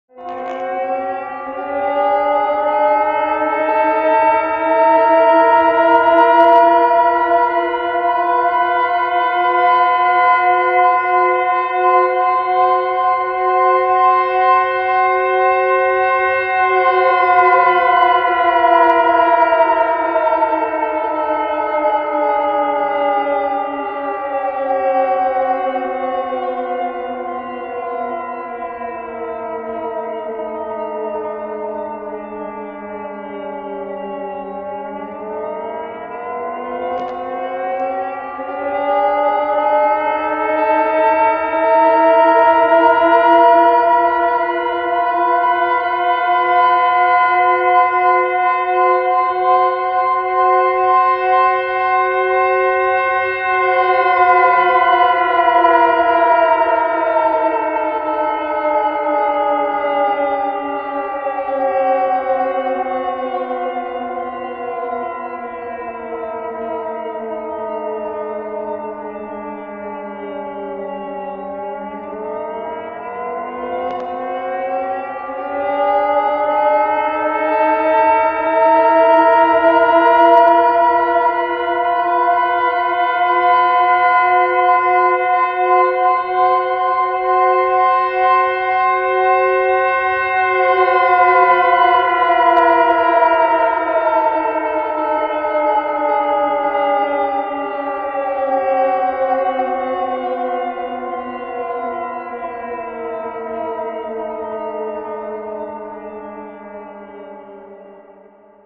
Air Raid Siren - Royalty Free Sound Effects
I took this from Internet Archive and altered it to sound a bit more drawn out and ominous. I also used this in the short film "The Rise Of The Walkers" (now available at Vimeo On Demand). It's yours to use, royalty free, for your monetized projects. Enjoy!